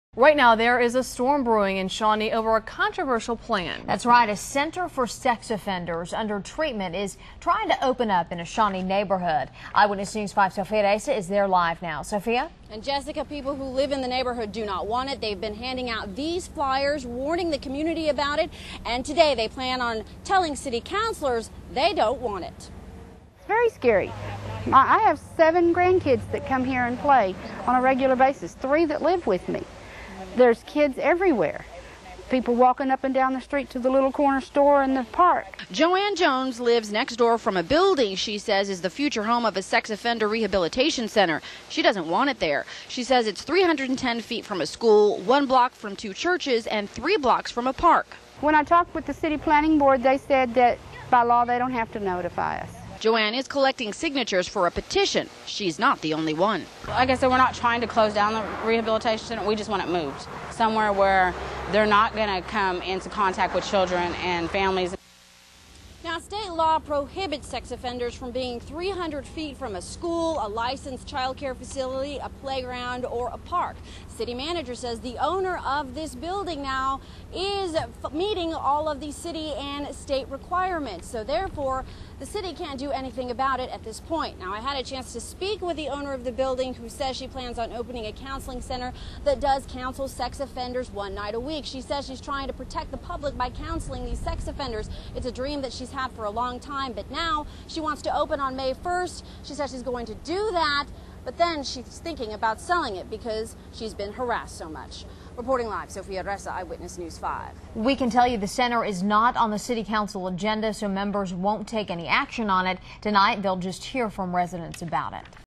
Neighborhood Speaks Out Against Shawnee Center
A sex offender rehabilitation center has a Shawnee neighborhood buzzing.